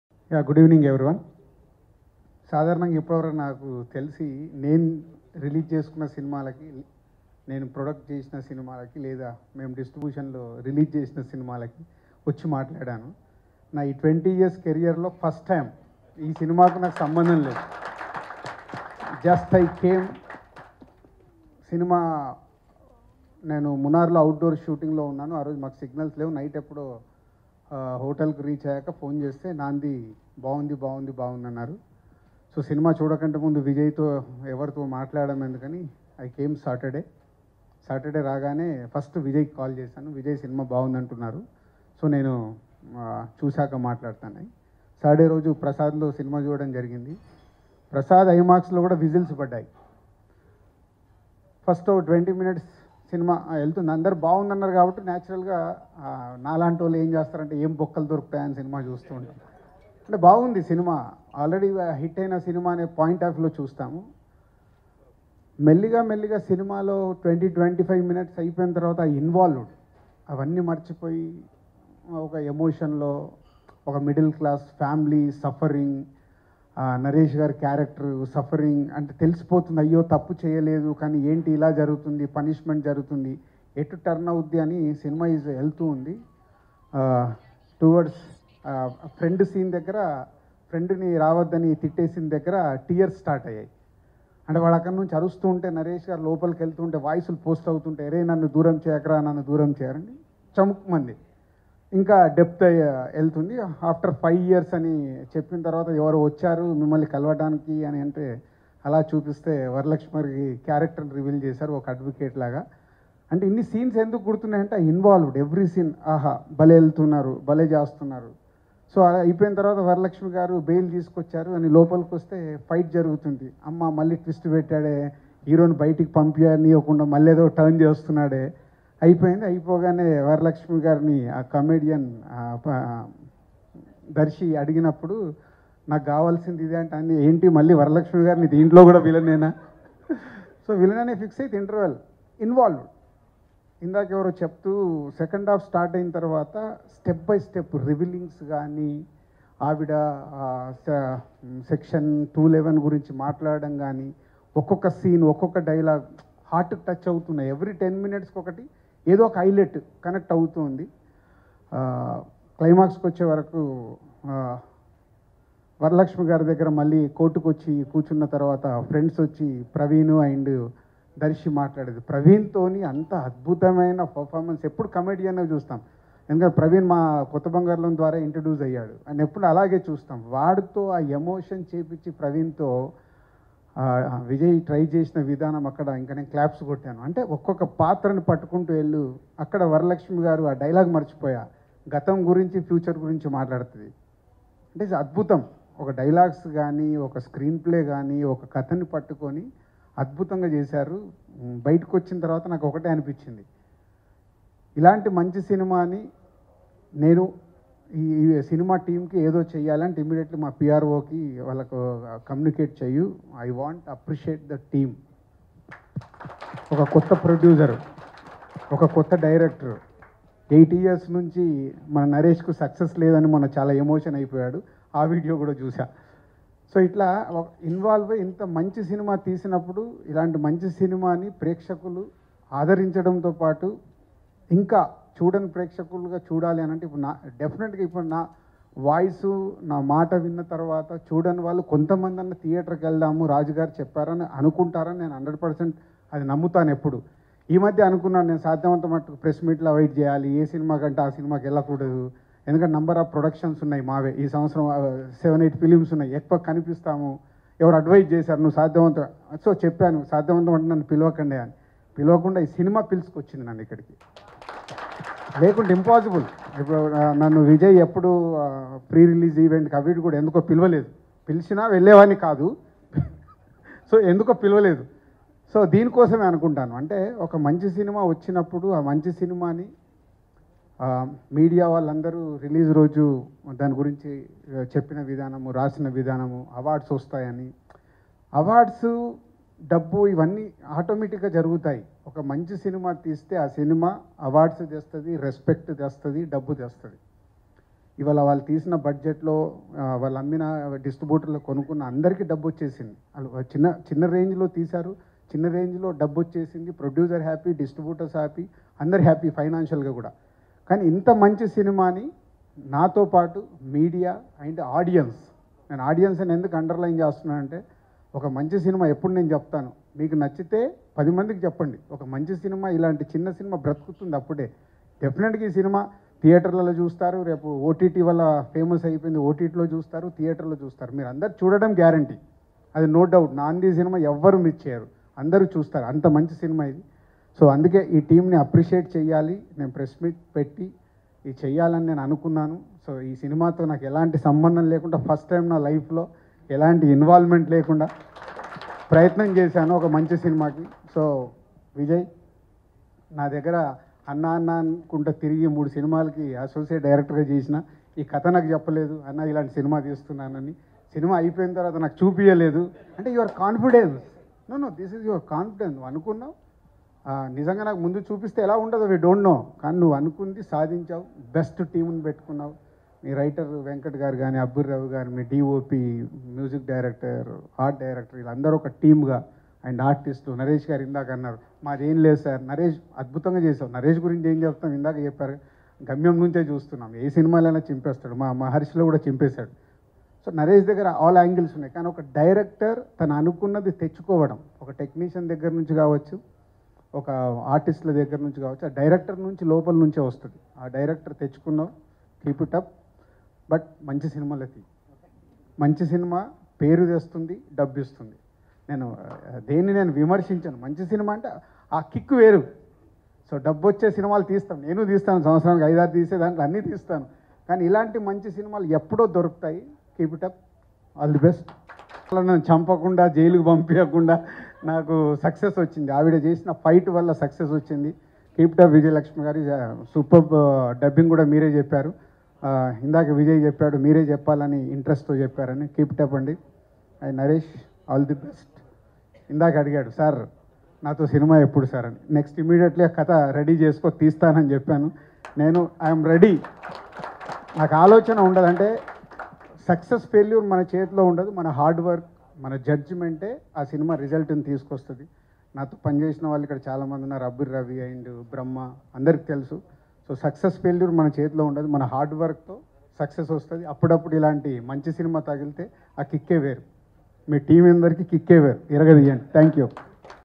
Producer Dil Raju Emotional Speech at Naandhi Team Appreciation Press Meet
Watch DilRaju​ Emotional Speech Naandhi Team Appreciation Press Meet

Team - Janam Swasa